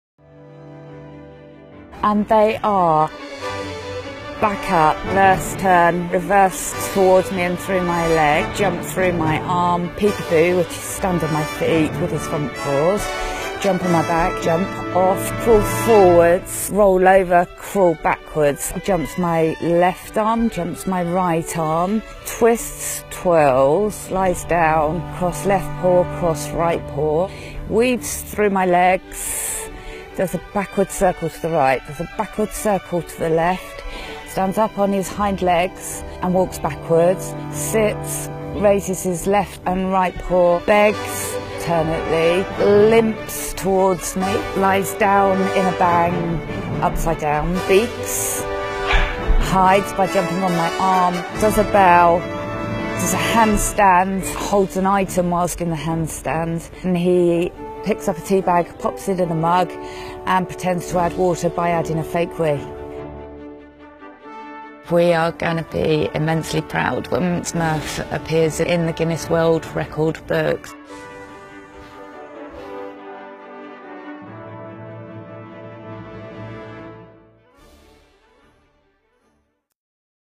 Introducing talented terrier Smurf (UK) who can perform an incredible 32 tricks in one minute!
Introducing talented terrier Smurf (UK) who can perform an incredible 32 tricks in one minute! This dynamic doggie stars in our new GWR2017 book, out now.